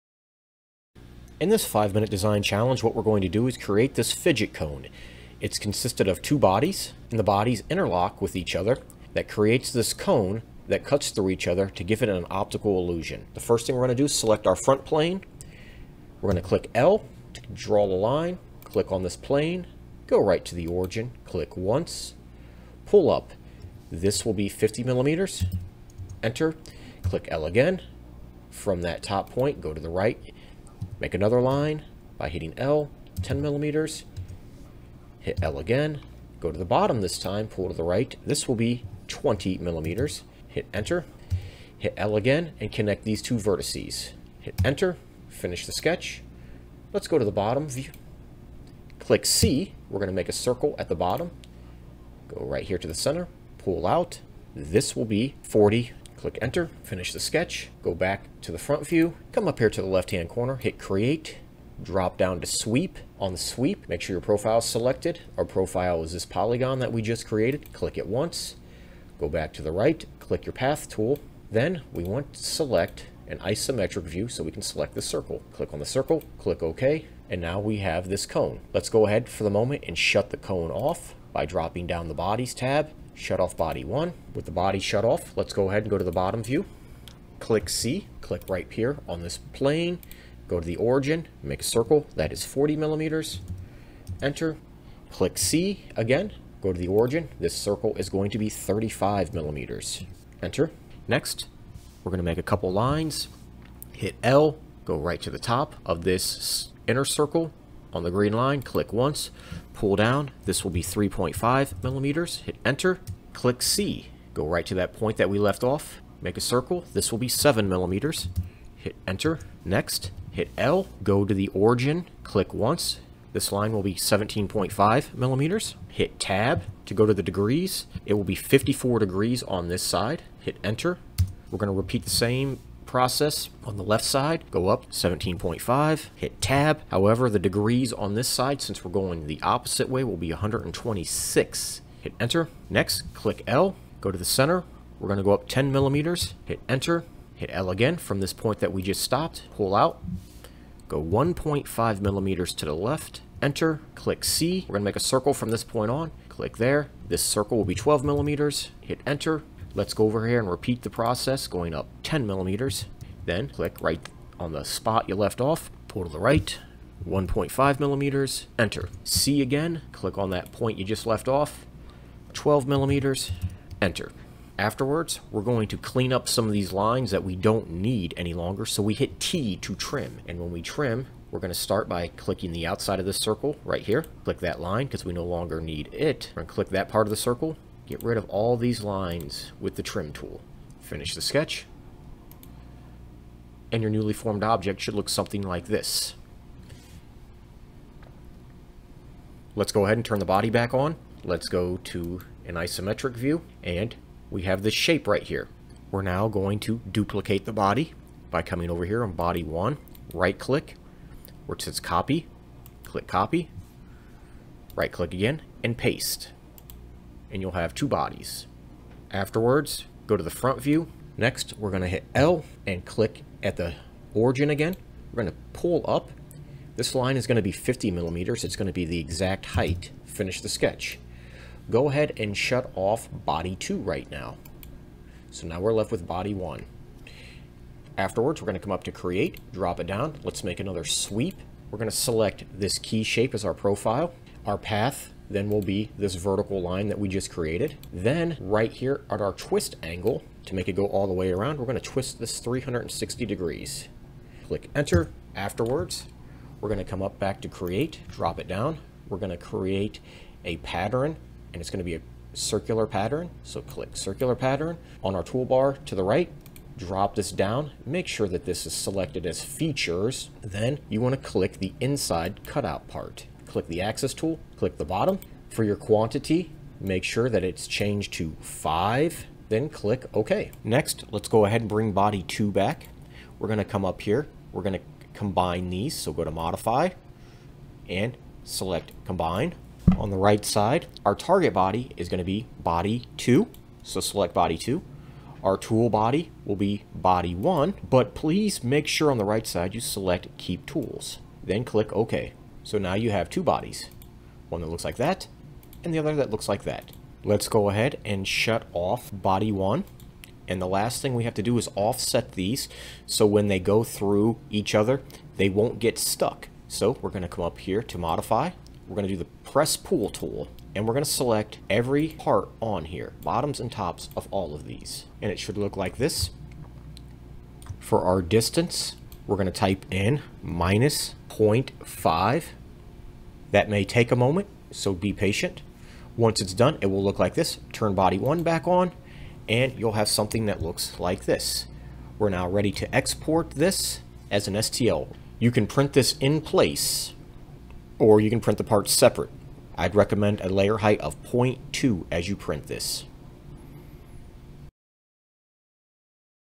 Fidget Cone Five Minute Design
Design a fidget cone in 5 minutes using Fusion 360;